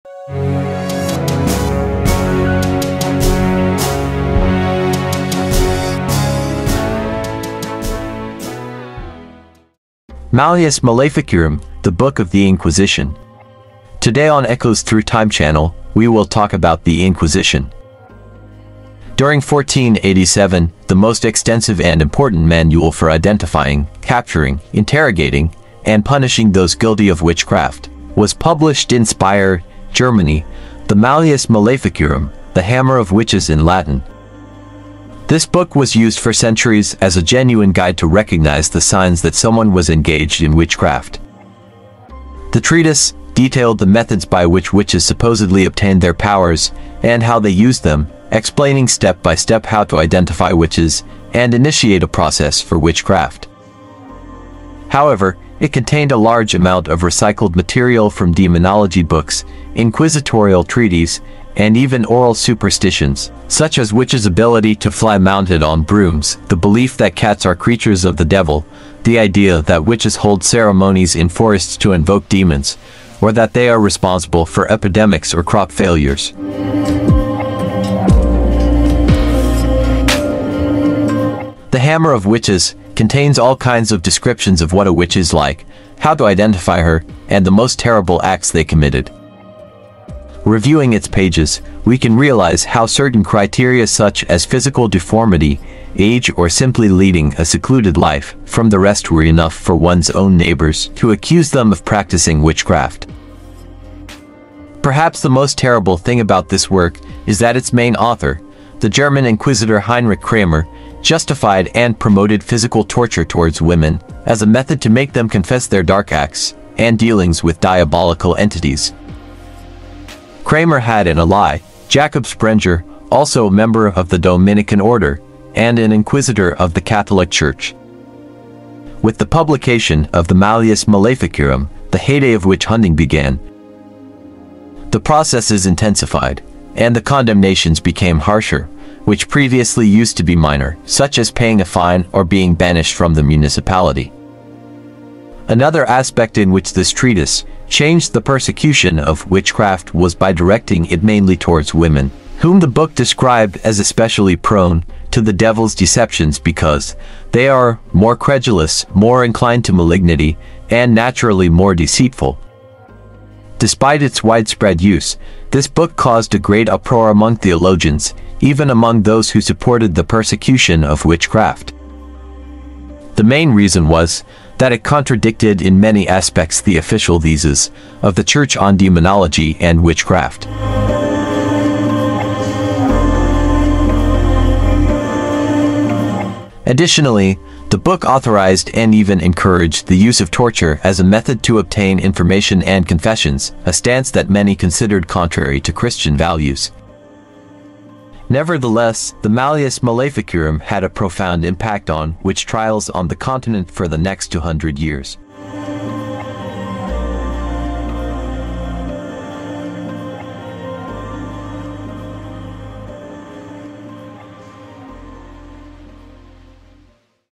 Malleus Maleficarum (The Inquisition Book)
Unearth the secrets of the 'Malleus Maleficarum' and its role in the witch hunts.
Echoes Throught Time Channel.